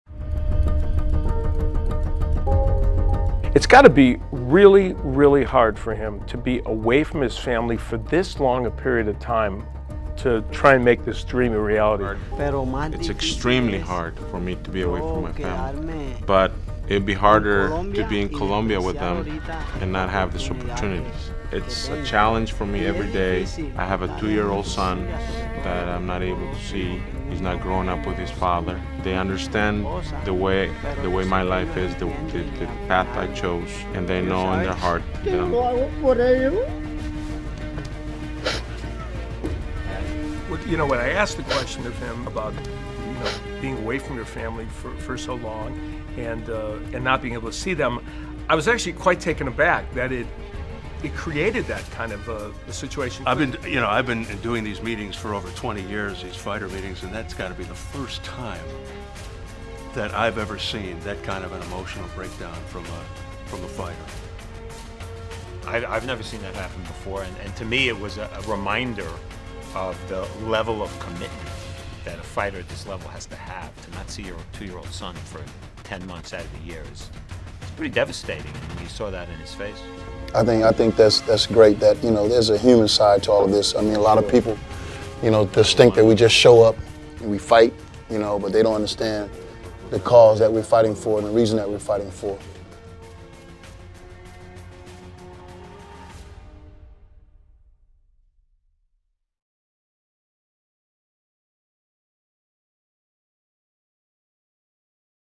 Yonnhy Perez - Bantamweight's Emotional Story of Sacrifice for Boxing - SHOWTIME Sports
In order to feed his family, IBF bantamweight champ Yonnhy Perez must be away from his loved ones ten months a year.

Training in America, Perez hardly sees his wife and kids back home in their downtrodden section of Colombia. While meeting with Showtime broadcasters in advance of tomorrow's bantamweight tournament kickoff, Perez was asked how he feels about training away from his family in order to be a champion and support them. He began to answer and then broke down sobbing. This rarely seen side of boxers exemplifies the sacrifices and struggles, not only to succeed in the ring, but to survive in life.